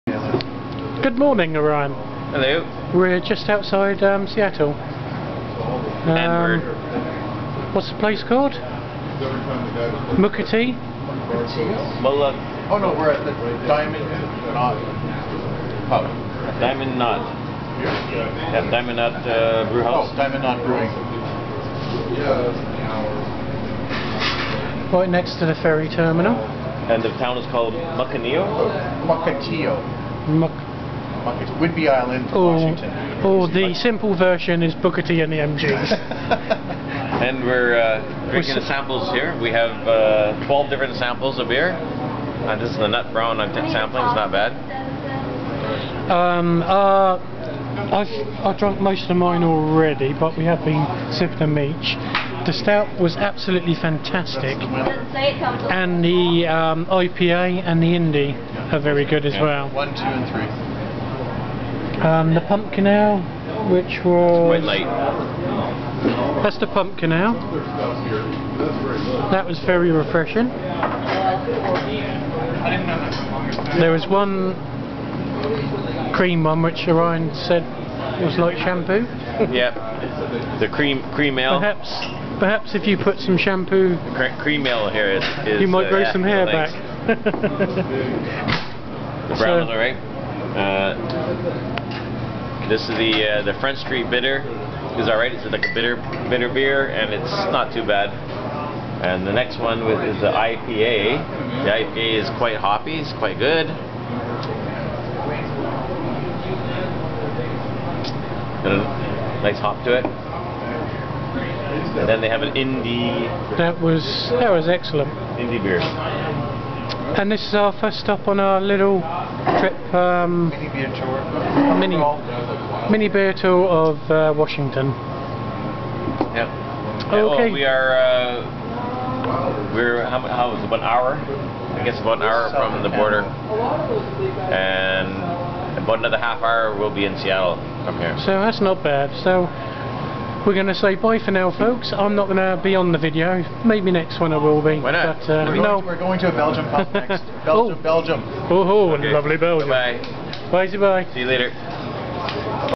Diamond Knot Brewing Company Washington
Marty and the boys mini trip to brewpubs washington state